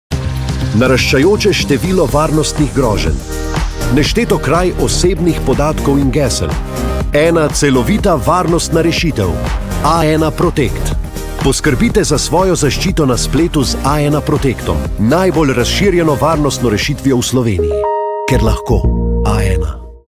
Poskrbite za varnost na spletu!
Do spleta večinoma dostopamo z mobilnih naprav, zato so te izjemno izpostavljene najrazličnejšim kibernetskim napadom, ki lahko vplivajo na delovanje naprave ali pa z nje pridobijo vaše osebne in druge podatke.

Uporabniki si sami redko kdaj vzamemo čas, da bi poskrbeli za spletno varnost naših naprav ter podatkov na teh, zato smo razvili storitvi A1 Protekt+ in A1 Protekt.